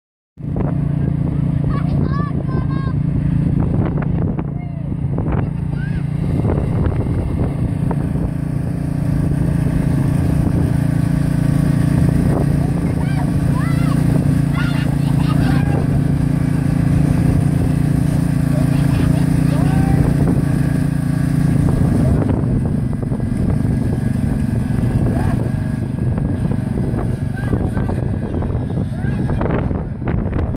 The Grass lads with Stephen Murray have more fun.
John Deere lawn mower tips Kane trailer and conal holds on.👍